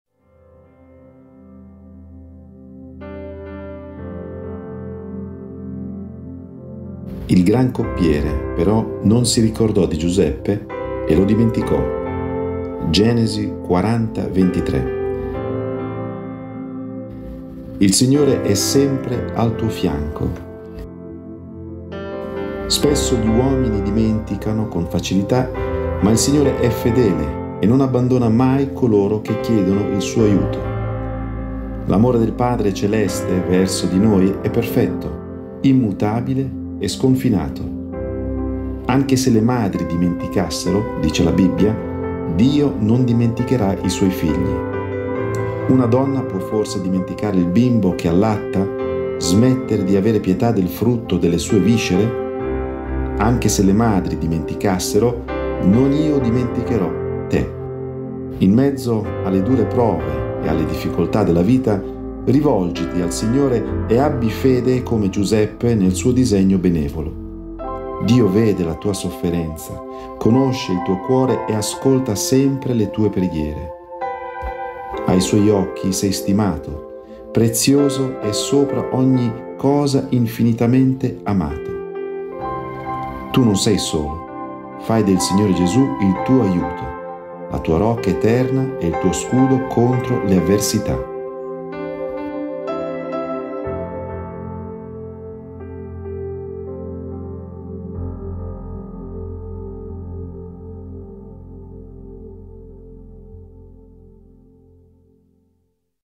"Il Signore è sempre al tuo fianco" - Meditazione - Genesi 40:23
"Il gran coppiere però non si ricordò di Giuseppe e lo dimenticò" (Genesi 40:23).

IL SIGNORE È SEMPRE AL TUO FIANCO

Spesso gli uomini dimenticano con facilità, ma il Signore è fedele e non abbandona mai coloro che chiedono il Suo aiuto. L'amore del Padre Celeste verso di noi è perfetto, immutabile e sconfinato. Anche se le madri dimenticassero, dice la Bibbia, Dio non dimenticherà i Suoi figli: "Una donna può forse dimenticare il bimbo che allatta, smettere di avere pietà del frutto delle sue viscere? Anche se le madri dimenticassero, non io dimenticherò te" (Isaia 49:15). In mezzo alle dure prove e alle difficoltà della vita, rivolgiti al Signore e abbi fede come Giuseppe nel Suo disegno benevolo. Dio vede la tua sofferenza, conosce il tuo cuore e ascolta sempre le tue preghiere. Ai Suoi occhi sei stimato, prezioso e sopra ogni cosa infinitamente amato. Tu non sei solo! Fai del Signore Gesù il tuo aiuto, la tua rocca eterna e il tuo scudo contro le avversità.